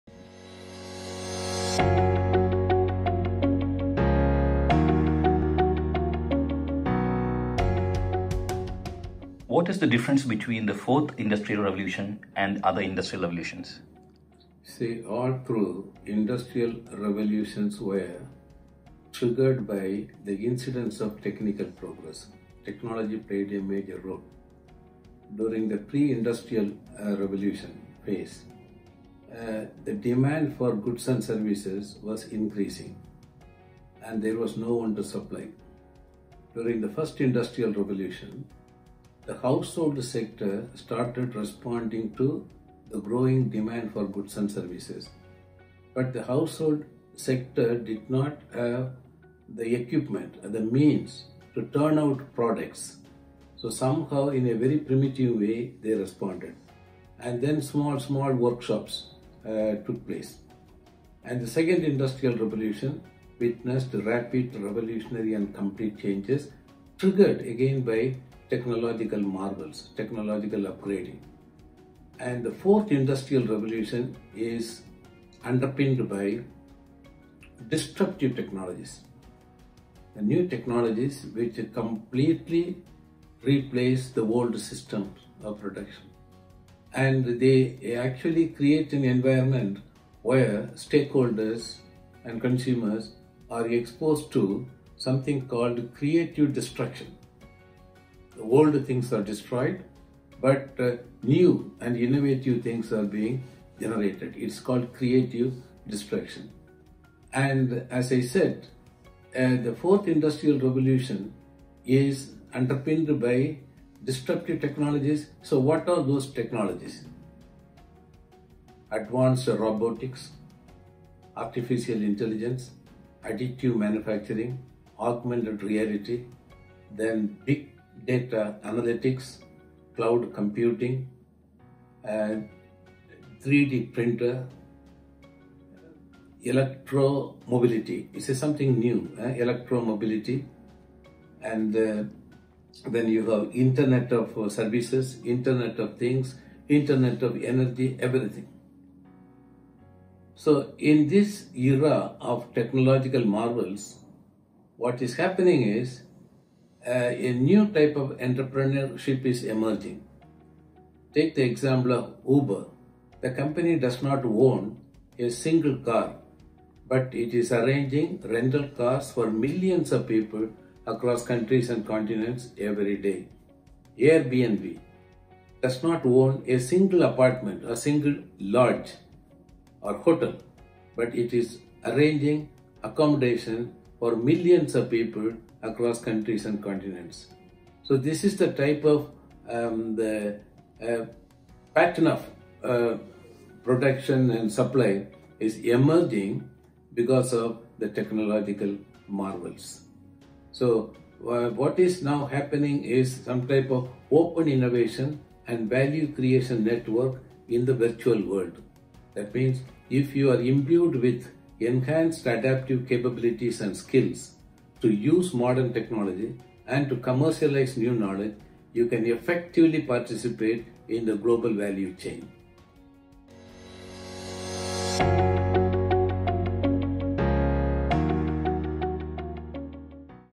Episode 5 | Difference between Industrial Revolutions | Dr Jebamalai Vinanchiarachi
Dr. Vinanchiarachi is a sustainable development expert with over 30 years of experience in the United Nations Industrial Development Organisation (UNIDO). He has authored several books and journal articles on the changing patterns of development, focusing on the key determinants of economic efficiency, ecological compliance, and social inclusion.